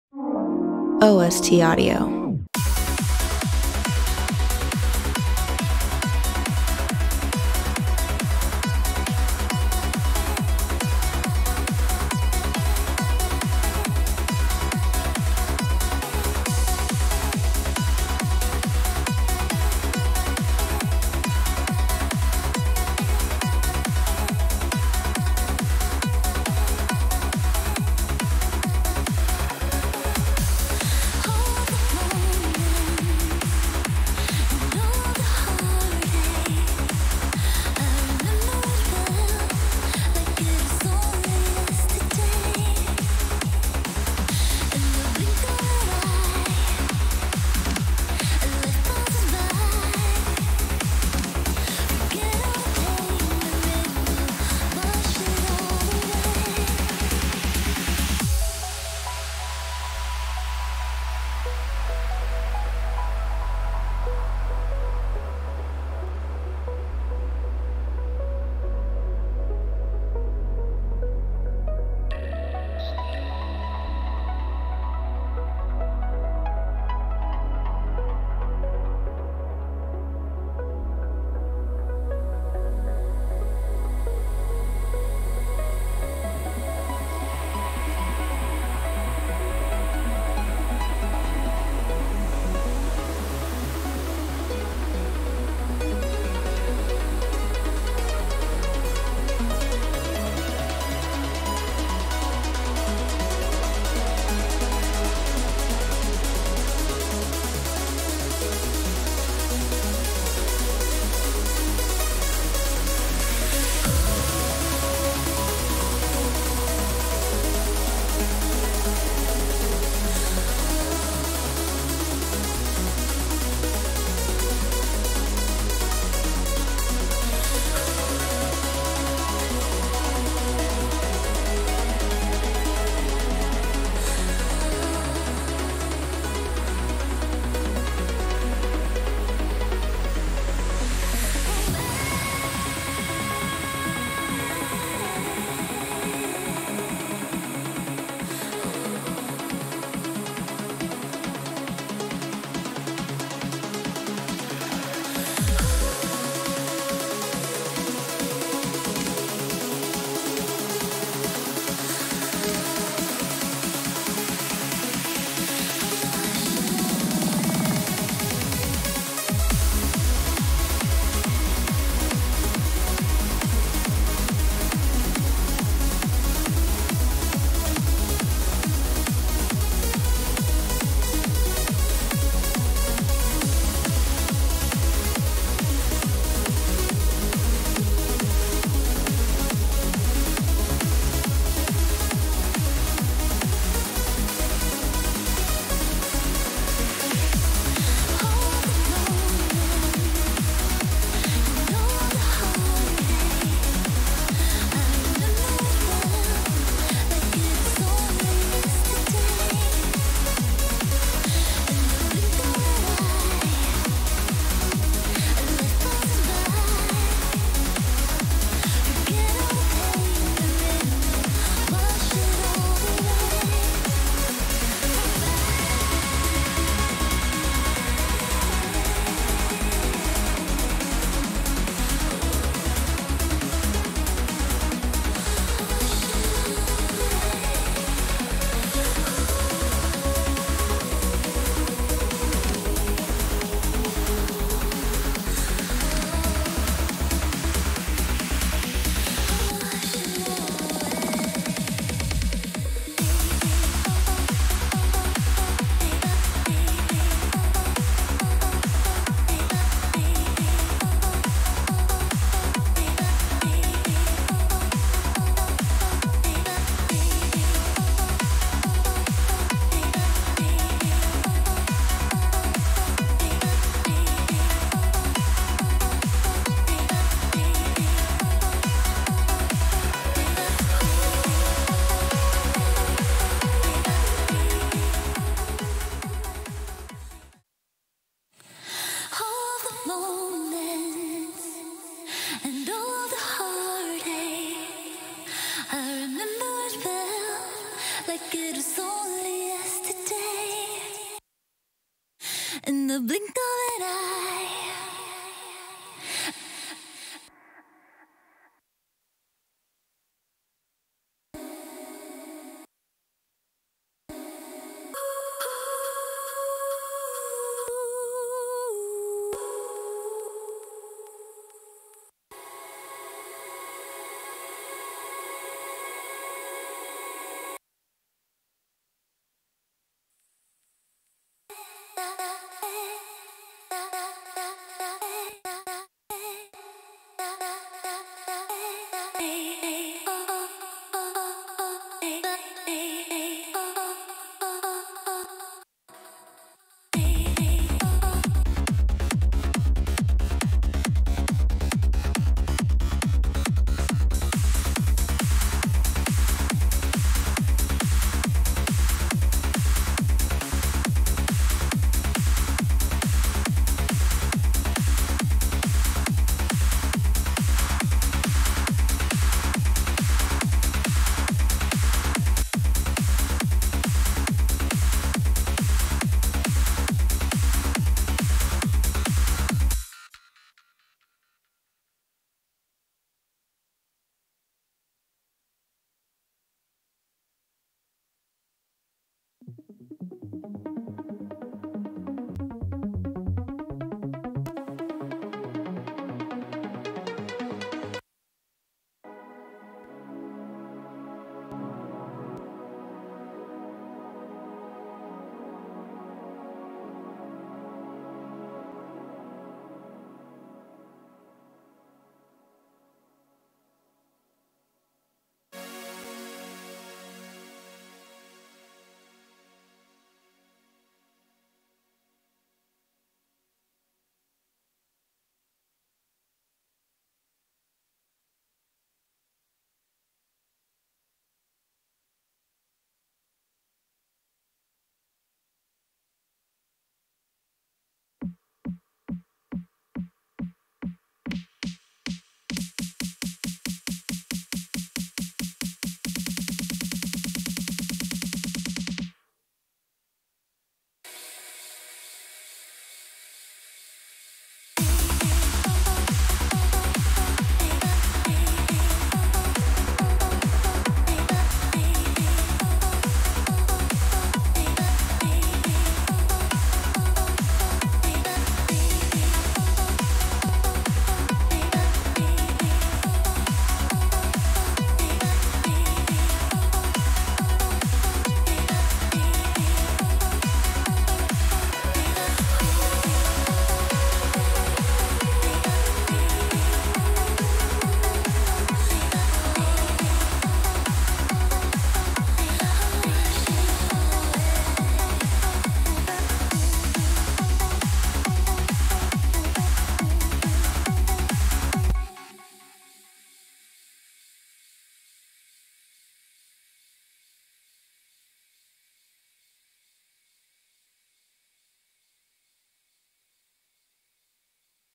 Vocal Trance Template With Rebecca Louise Burch FL Studio
Hello guys! together with Rebecca, we created this project to help you learn professional vocal trance production.
This template is a complete tool that will show you every step to achieving professional sound, vocal mixing, trance track mixing, sound design, mastering, and more.